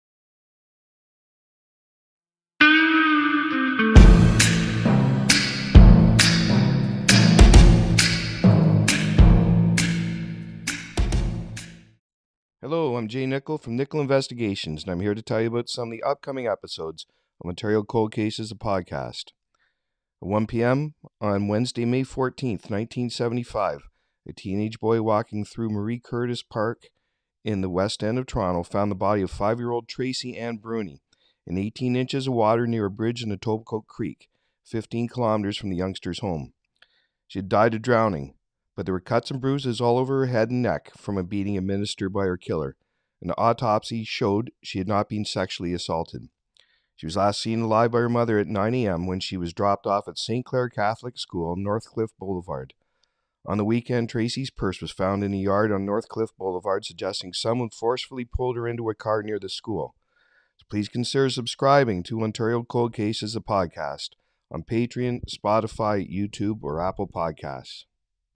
Ontario Cold Cases - The Podcast: Upcoming Episode! Tracy Bruney
At 1 p.m. on Wednesday, May 14th, 1975, a teenage boy walking through Marie Curtis Park found the body of 5-year-old Tracey Ann Bruney in 18 inches of water near a bridge in Etobicoke Creek, 15 km from the youngster’s home.

She had died of drowning, but there were cuts and bruises all over her head and neck from a beating administered by her killer. An autopsy showed she had not been sexually assaulted.

She was last seen alive by her mother at 9a.m., when she was dropped off at St. Clare Catholic School on Northcliffe Blvd. On the weekend, Tracey’s purse was found in a yard on Northcliffe Blvd., suggesting someone forcefully pulled her into a car near the school.

Fundraiser by John Nicoll : Ontario Cold Cases - The Podcast